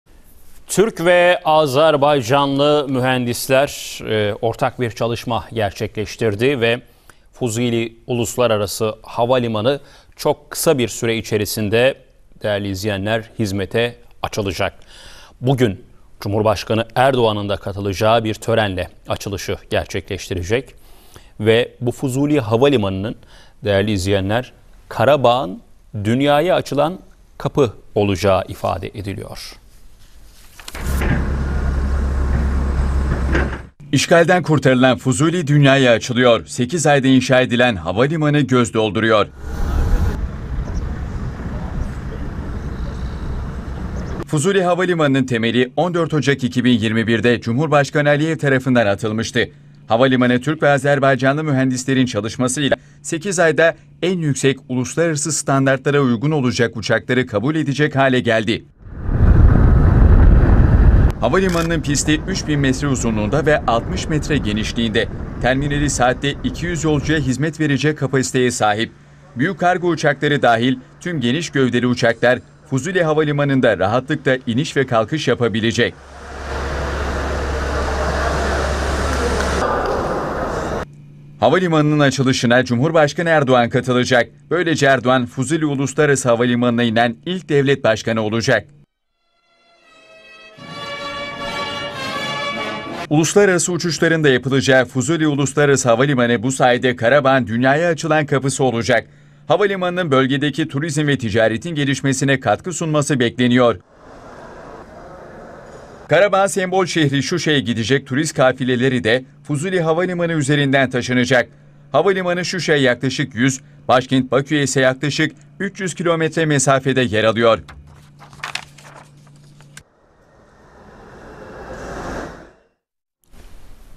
Erdoğan ve Aliyev'in açılışını yapacağı Fuzuli Havalimanı 8 ayda inşa edildi
Azerbaycan'ın işgalden kurtardığı topraklarda inşa ettiği ilk havalimanı Cumhurbaşkanları Erdoğan ve Aliyev'in katılacağı törenle hizmete girecek. Cumhurbaşkanı Erdoğan, Fuzuli Uluslararası Havalimanı'na inen ilk devlet başkanı olacak. Temeli 14 Ocak 2021'de Cumhurbaşkanı Aliyev tarafından atılan Fuzuli Havalimanı en yüksek uluslararası standartlara uygun olarak 8 ayda uçakları kabul edecek hale geldi. Havalimanına ilk test uçuşu 22 Ağustos'ta gerçekleştirildi.

Havalimanının pisti 3 bin metre uzunluğunda ve 60 metre genişliğinde. Terminali saatte 200 yolcuya hizmet verecek kapasiteye sahip. Büyük kargo uçakları dahil tüm geniş gövdeli uçaklar Fuzuli Havalimanı'nda rahatlıkla iniş ve kalkış yapabilecek.

Otomatik sistemlerle donatılmış, uluslararası sivil havacılık ve taşımacılık standartlarına uygun kontrol kulesine sahip havalimanının inşaatında Türk şirketleri de yer aldı.